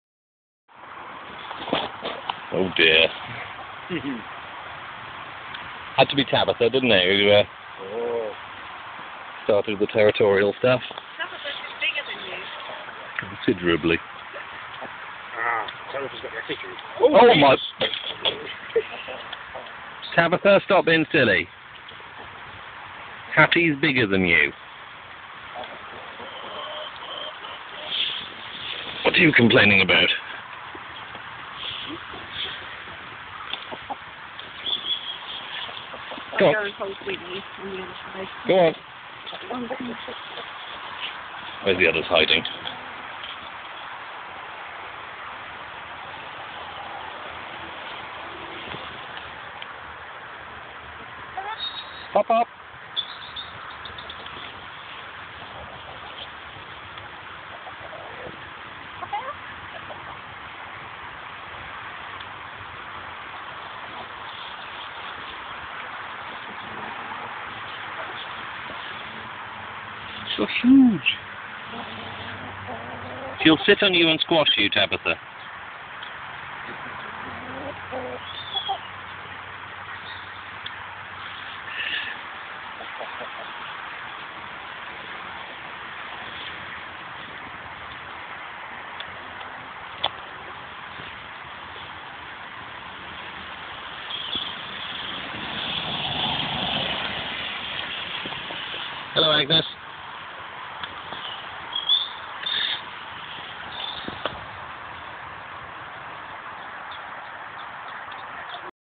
New chooks! TheChooks meet their new neighbours
4 new Chooks today! Bit of a territorial dispute but no more than was expected...